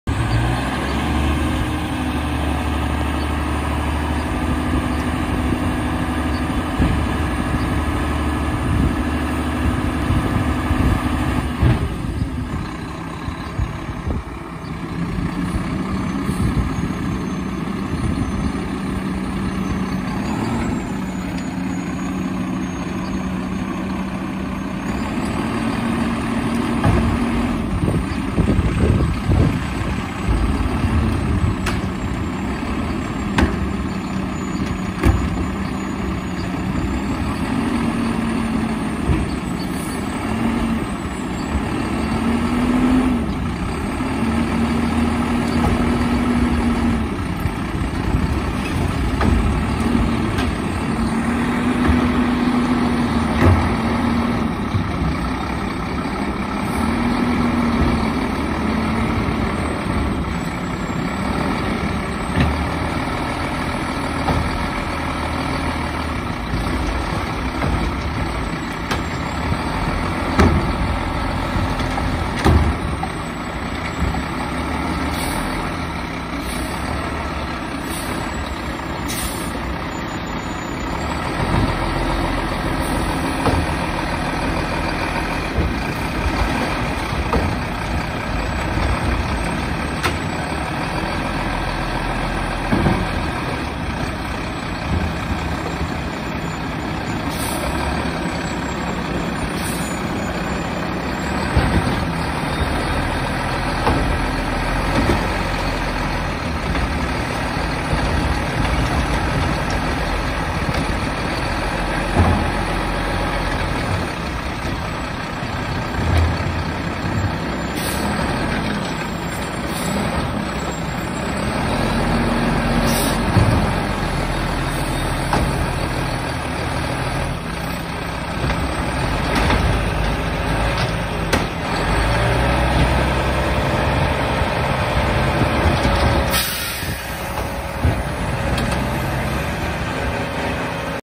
Republic services labrie on a recycle cart line.
2415 doing a recycling cart line at a apartment.